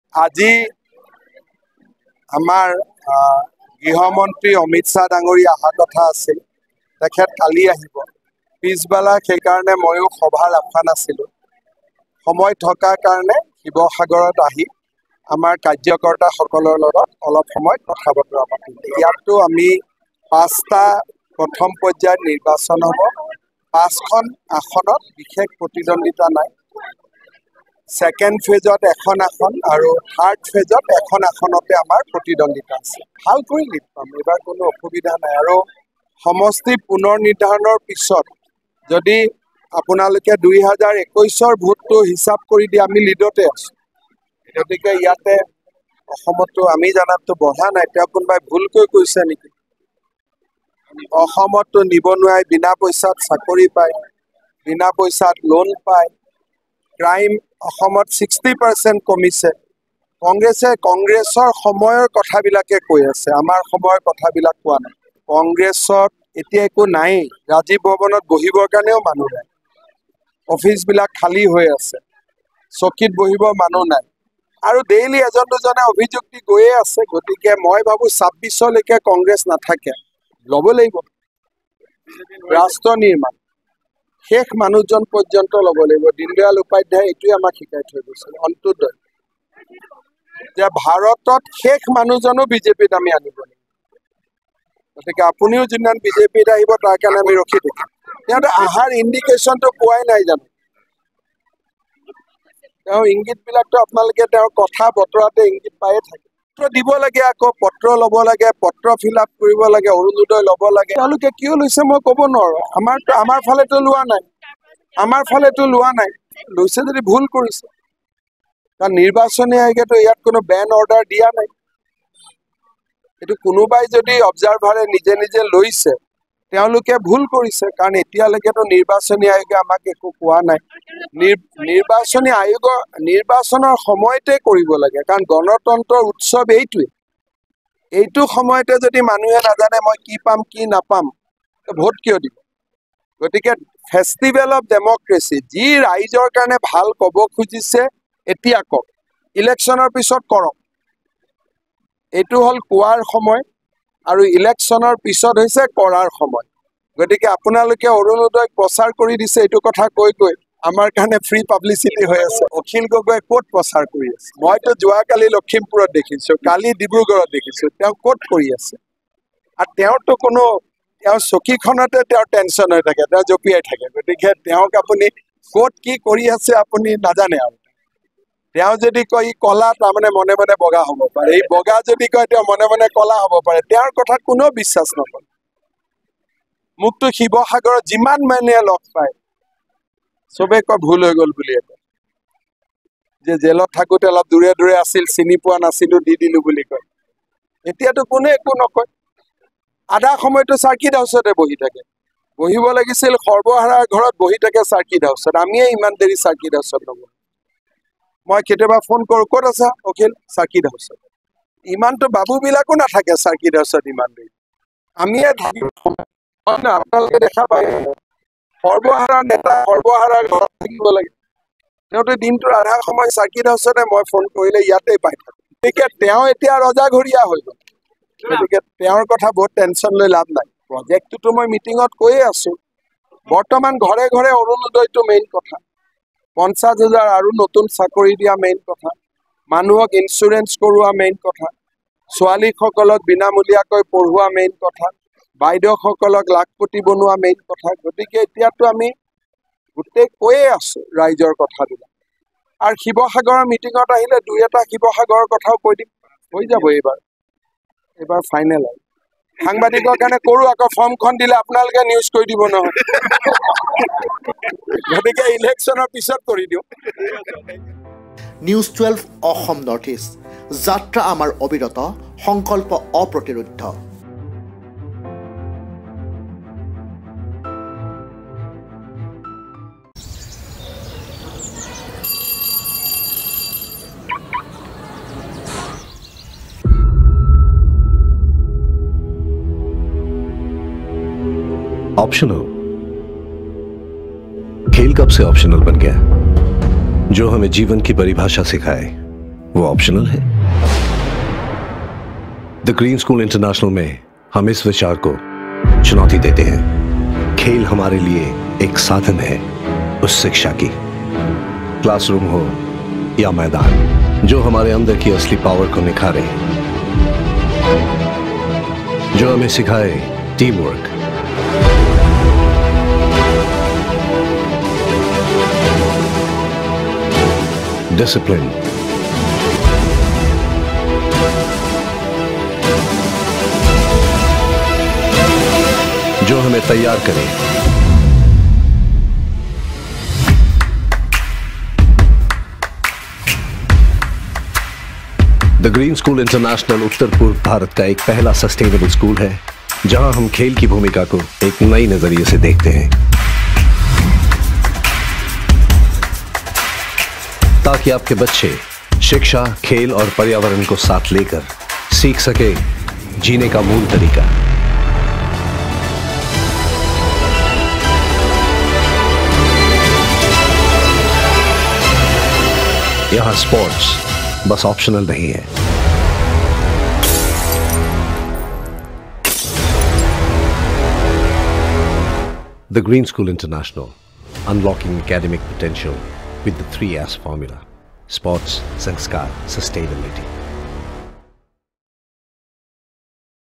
শিৱসাগৰত কি ক'লে মুখ্যমন্ত্ৰী ড০ হিমন্ত বিশ্ব শৰ্মাই | News 12 Assam North East
শিৱসাগৰত মুখ্যমন্ত্ৰী ড০ হিমন্ত বিশ্ব শৰ্মাৰ মন্তব্য॥ ভাৰতবৰ্ষৰ শেষৰজন ব্যক্তিও যেতিয়ালৈকে বিজেপিলৈ নাহে তেতিয়ালৈকে আমাৰ প্ৰচেষ্টা অব্যাহত থাকিবঃ মুখ্যমন্ত্ৰী

 Follow our